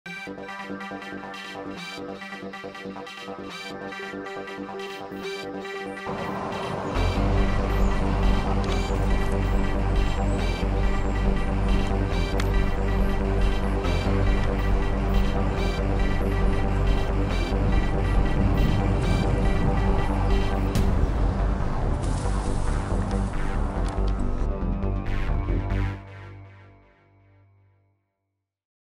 Crazy Squirrel Downtown
Squirrel downtown trying to escape the city life